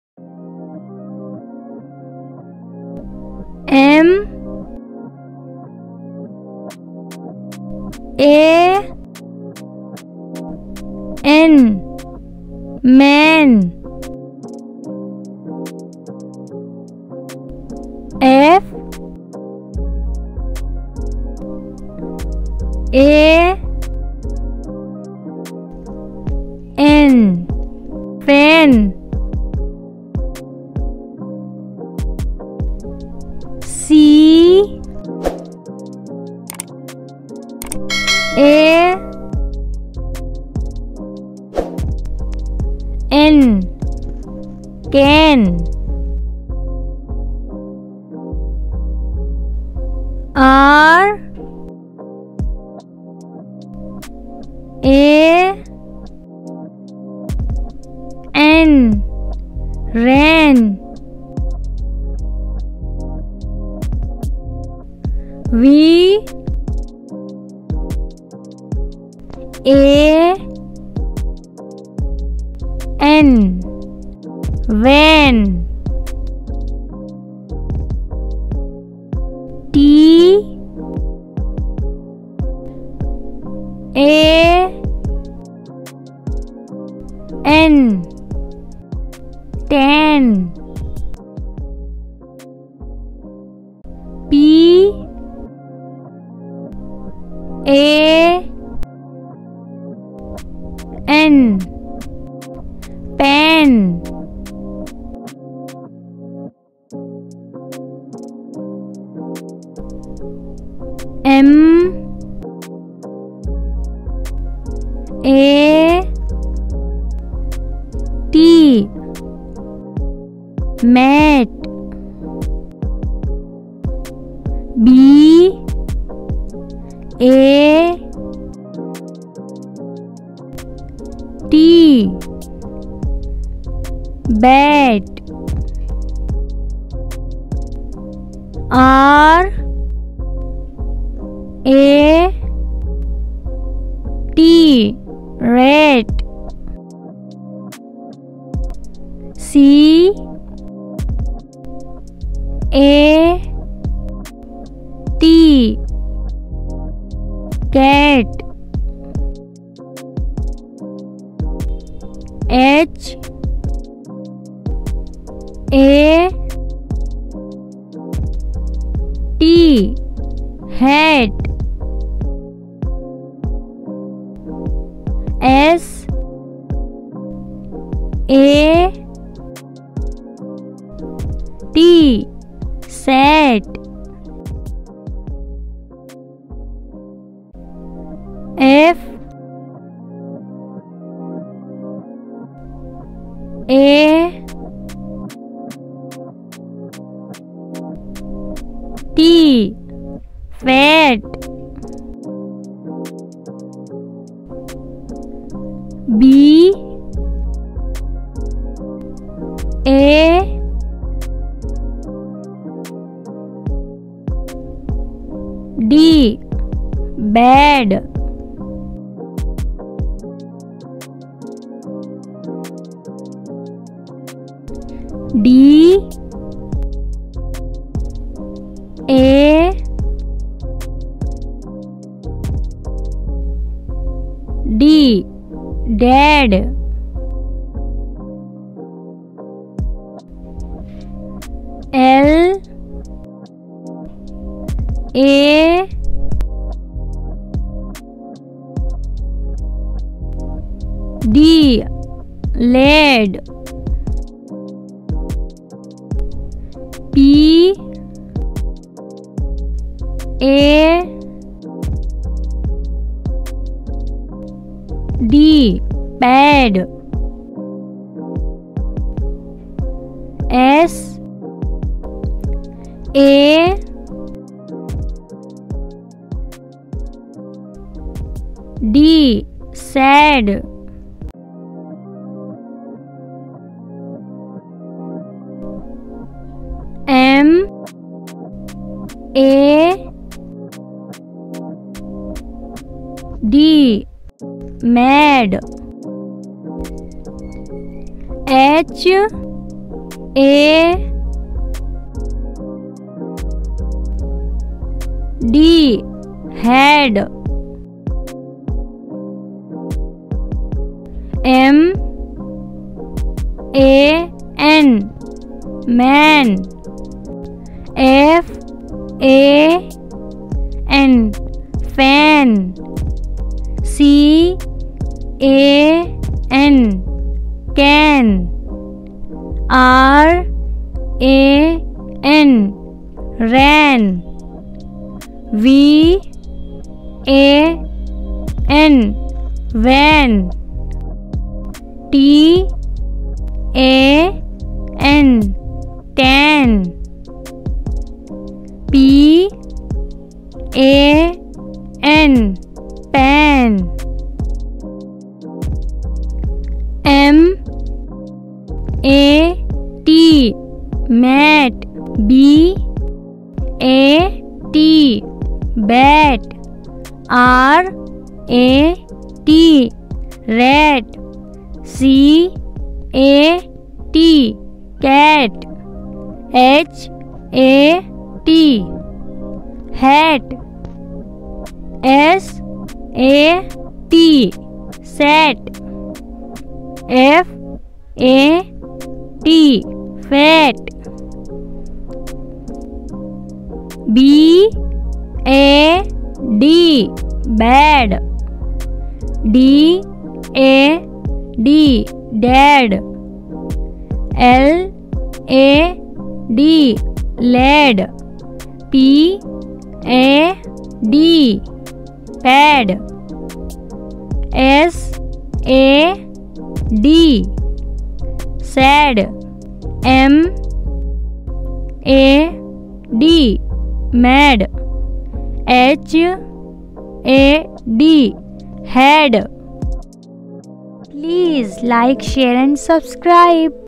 Three Letter Words l "A" Short Sound Words (an at ad ) sound of A ( A की ध्वनी )L.K.G. U.K.G PART 1
Three Letter Words ll A Short Sound Words (an at ad ) sound of A ( A ki dhavni ) L.K.G. U.K.G. PART (1)

Learn vowel "a" sound with short words sentences
Three letter words for kids learn how to read and write A sound words.